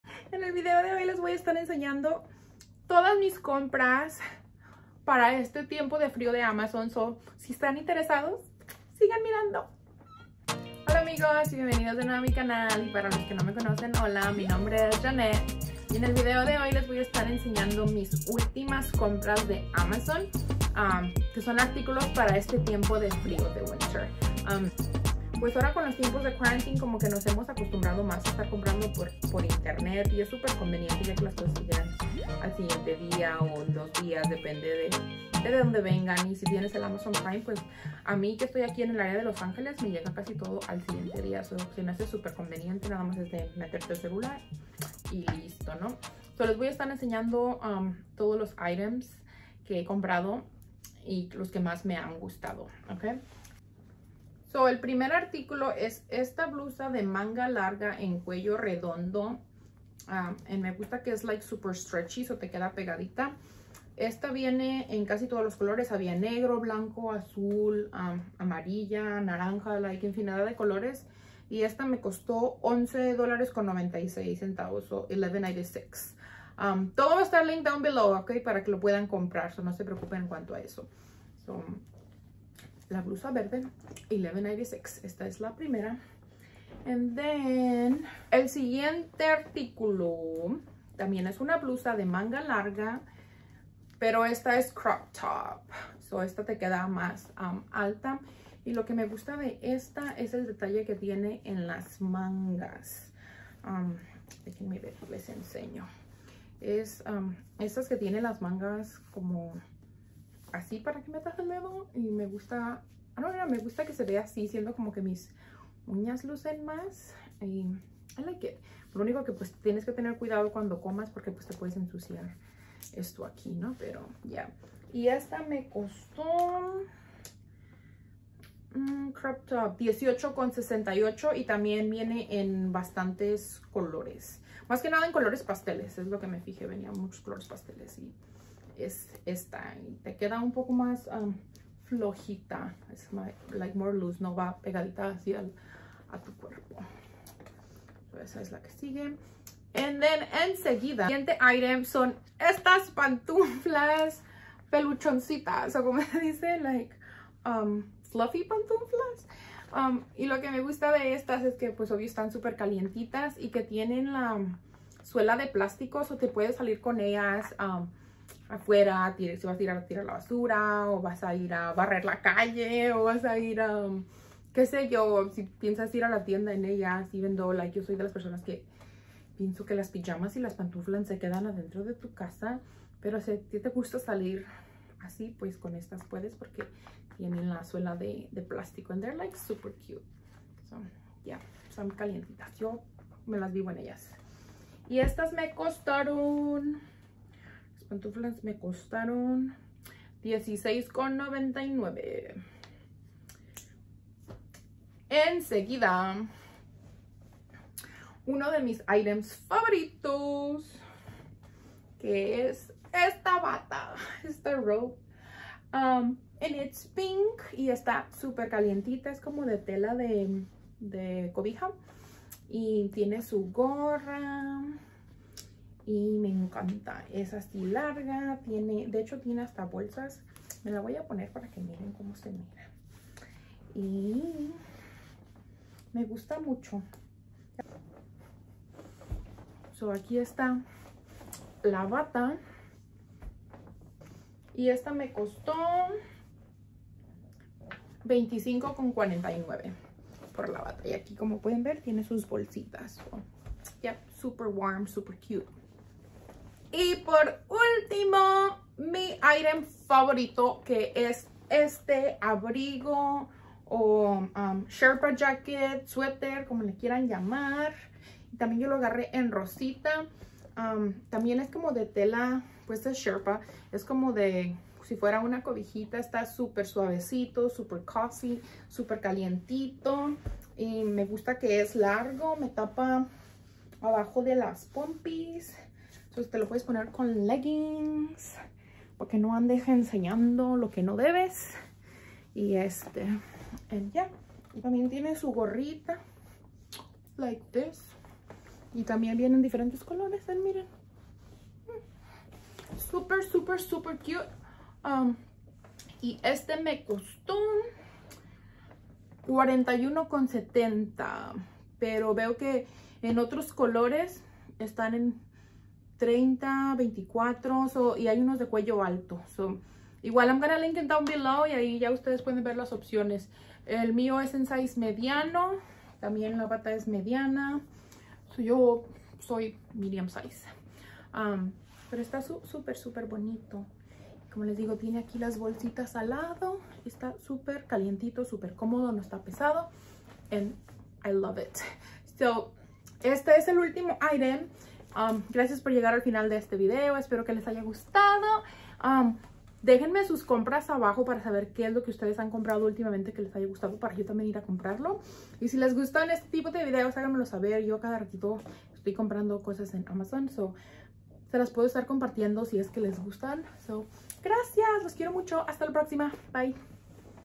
Que comprar en AMAZON Otoño/Invierno 2021 | Favoritos de AMAZON | Mira lo que COMPRE
Mis compras favoritas para este otoño/invierno de Amazon. Perfectas para  este frio y para regalos de navidad

Aquí están los links directos

Blusa manga larga verde

Blusa crop manga larga morada

Pantunflas

Bata/robe rosita

Sherpa sweater